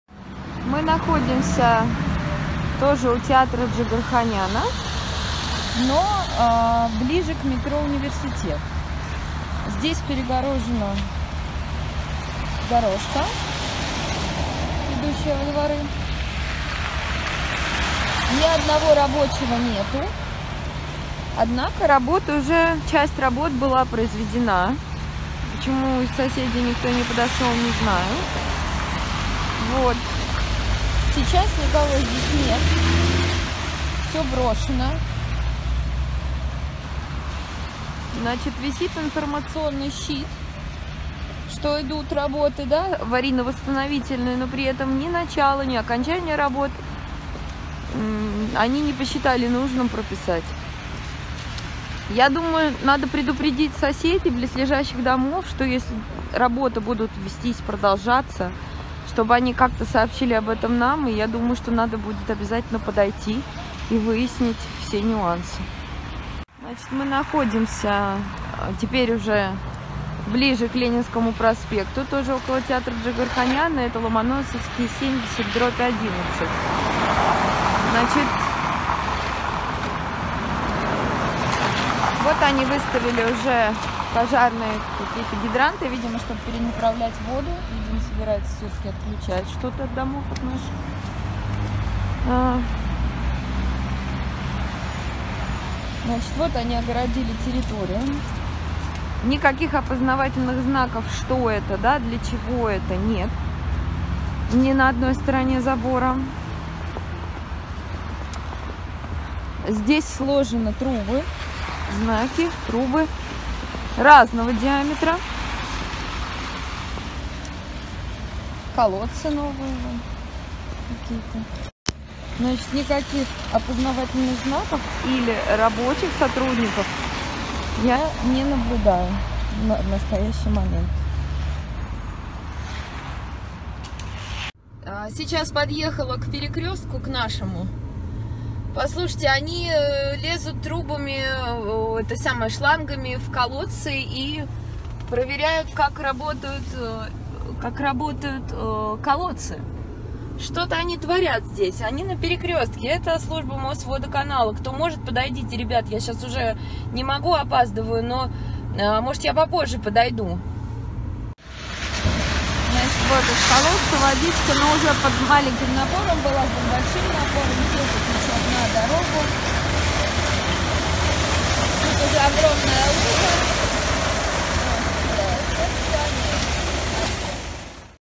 "Вирус Мосводоканала" в Гагаринском районе, Ломоносовский проспект. 31.01.2020
Внезапно по всему району начались "Аварийные работы" от "Мосводоканала". Из достоверных источников, от очевидцев и муниципальных депутатов стало известно, что работы ведутся с нарушениями и ведутся не в рамках "Аварийных работ", а идёт перекладка коммуникаций под кусок ветки метро из "Коммунарки" от ул.Новаторов до Севастопольского проспекта. Везде идут работы с нарушением ППМ 299-ПП, а также без положительного заключения Мосгосэкспертизы, работы ведутся по госзакупкам не связанными с "Аварийными работами".
Жители, будьте бдительны, не проходите мимо опасных и незаконных работ, вызывайте полицию, требуйте оригиналов документов на "аварийные работы"!